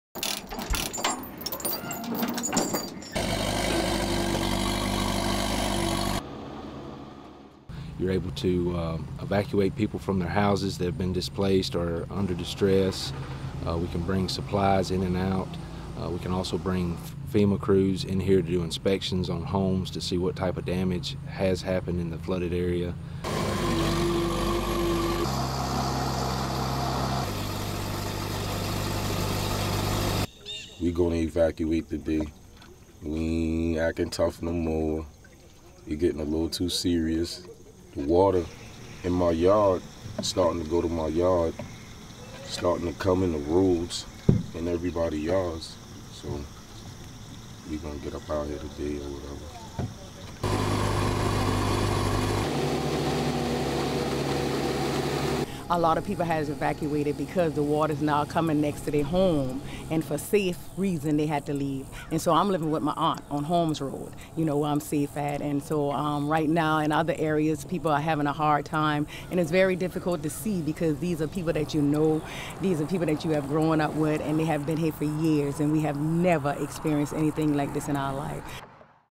S.C. Residents Evacuate Flooded Neighborhoods
(10 Oct 2015) FOR CLEAN VERSION SEE STORY NUMBER: apus040021

Nearly a week after the heavy rains brought record flooding to South Carolina, people are still evacuating the lowcountry coastal region as flood waters continue to rise.
On Saturday, residents of the Brown Ferry community were being assisted with their evacuation efforts by a Hydratrek amphibious crew contracted by the Federal Emergency Management Agency.
Flood waters near the Black River continued to rise and cause more residents to second guess there determination to ride out the devastation.
Officials in Georgetown say the Black River crested overnight and water levels have dropped 5 inches.
Georgetown County spokeswoman Jackie Broach said Saturday the worst flooding appears to be over for some parts of the county.
Record rainfall dumped as much as 20 inches of rain in Columbia. That rain has swollen rivers past their banks as the water flowed to the coast, threatening coastal cities with more flooding.
The Waccamaw River crested Friday and began to recede.
The Black River soon followed. Both rivers empty into Winyah Bay and the Atlantic ocean at Georgetown.
The National Guard went from house to house on Friday urging people to evacuate, but many chose to stay.